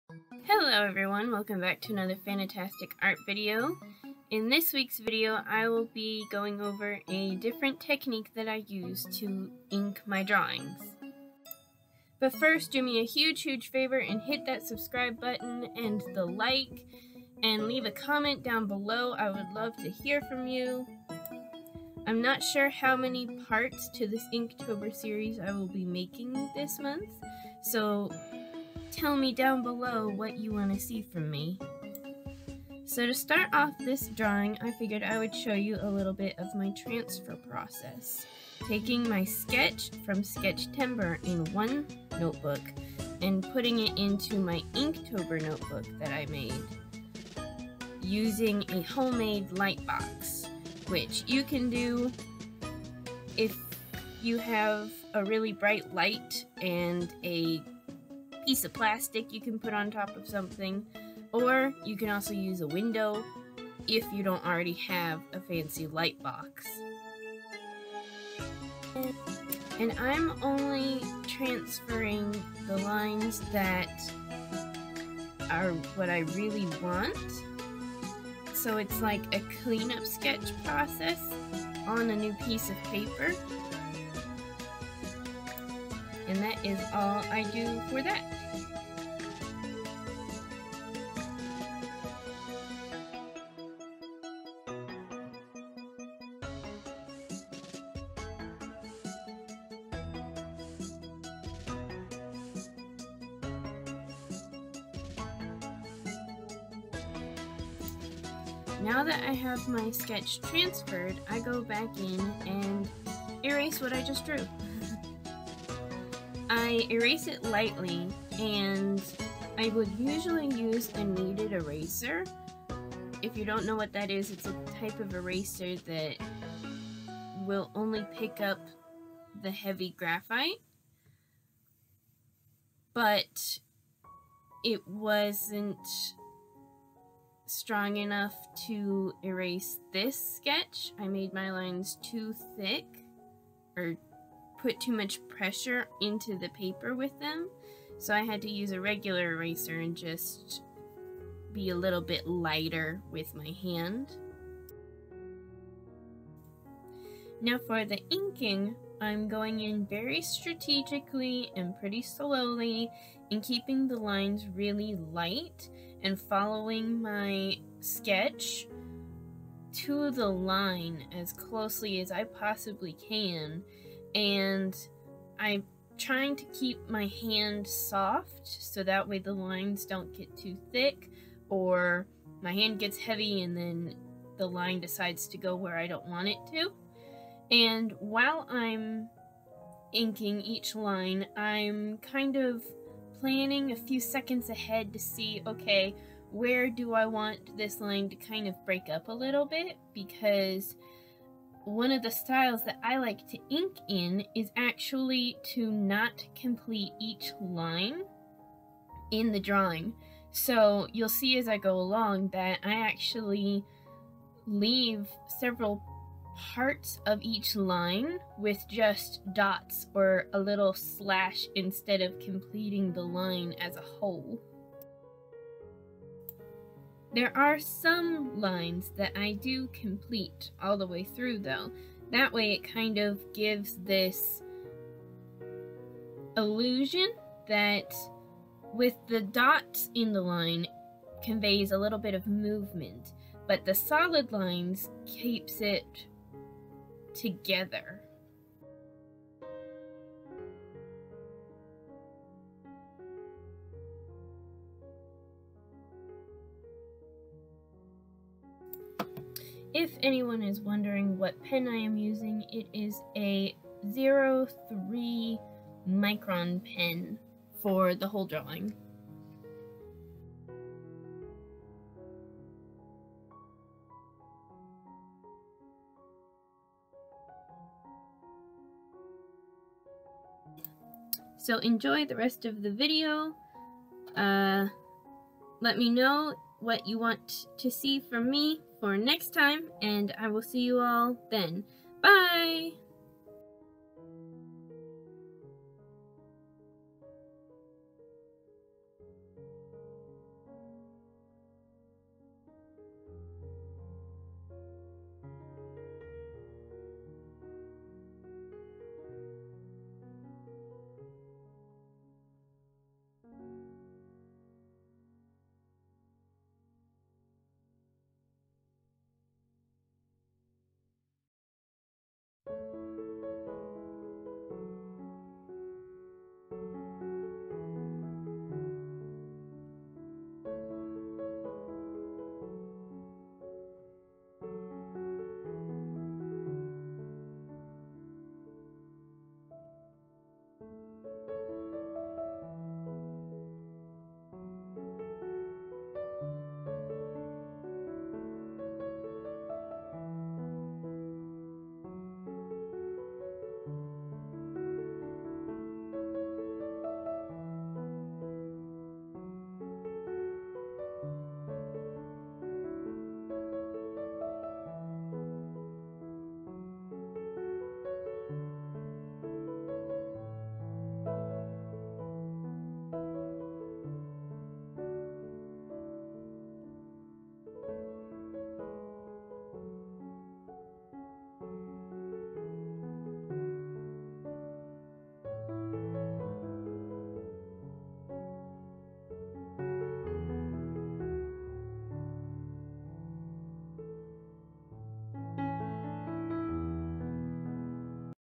Inktober 2021 |Part 3
Hi everyone! In this week's video I will be sharing my transfer process from sketchtember into my inktober notebook. Then I hope you will ink along while I show you my favorite way to ink characters that need that little bit of movement or extra flow!